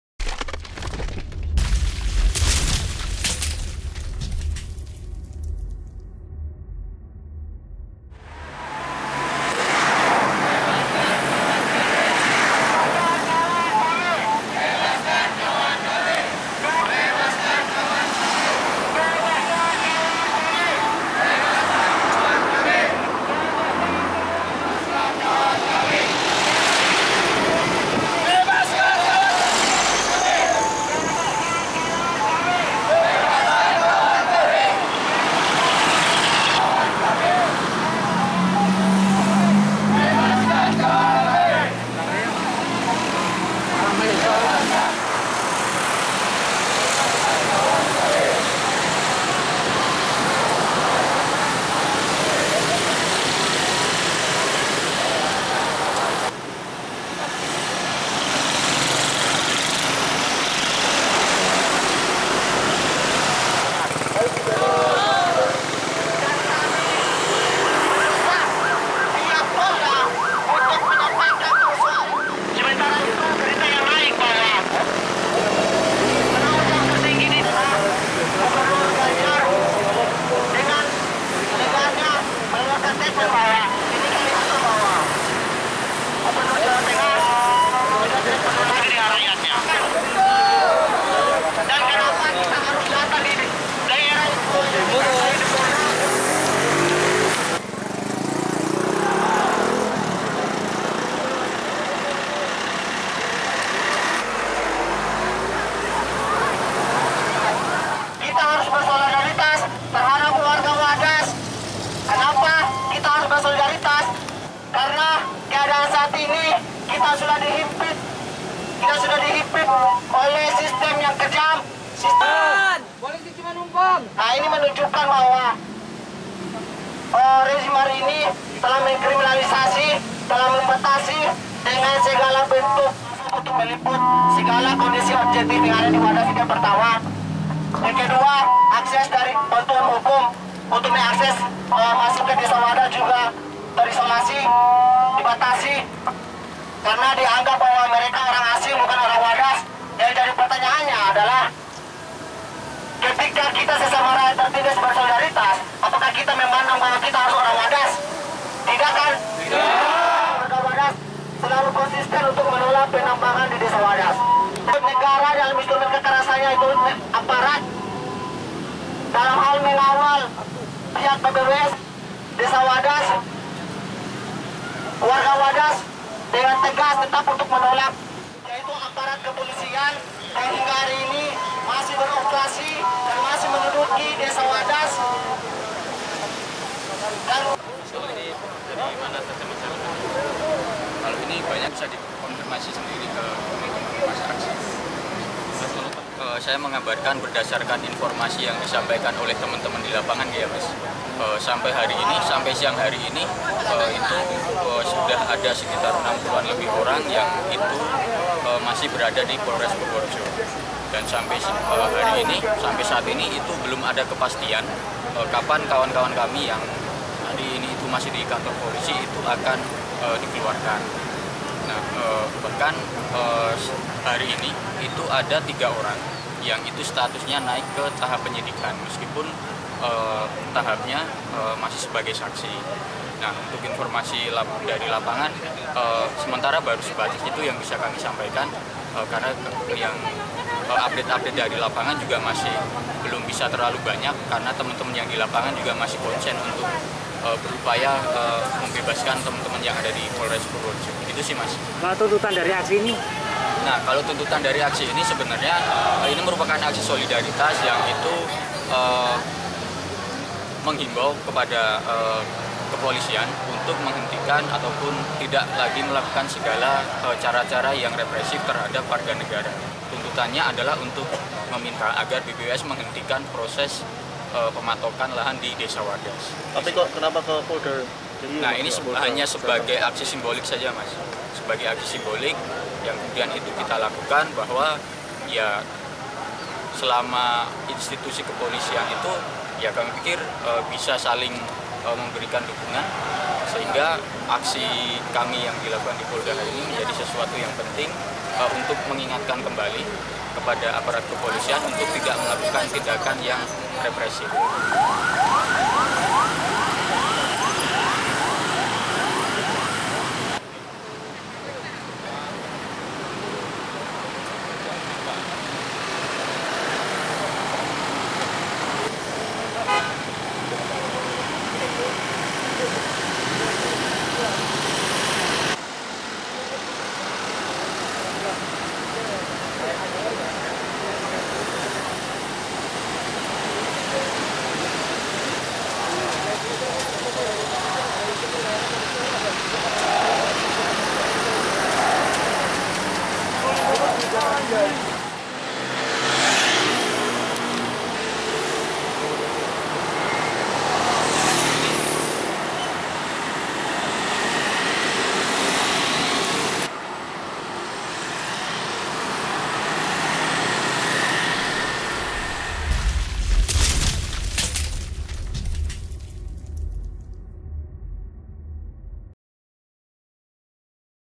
SOLIDARITAS UNTUK WADAS; RATUSAN MASSA GERUDUK & DEMO POLISI
Massa yang tergabung dalam Aliansi Solidaritas untuk Wadas mendatangi Polda DIY dan kantor Balai Besar Wilayah Sungai (BBWS) Serayu-Opak, Rabu (9/2/2022).

Mereka melakukan aksi solidaritas atas adanya aksi kekerasan dan penangkapan yang dilakukan oleh polisi terhadap warga Wadas.

Kepala Divisi Penelitian LBH Yogyakarta Era Hareva, yang juga tergabung dalam Aliansi Solidaritas, menjelaskan sampai siang hari ini sudah ada sekitar 60 lebih orang yang masih berada di Polres Purworejo.

Tuntutan dari aksi ini, kata dia, merupakan aksi solidaritas yang itu mengimbau kepada kepolisian untuk menghentikan ataupun tidak lagi melakukan segala cara-cara yang represif kepada warga.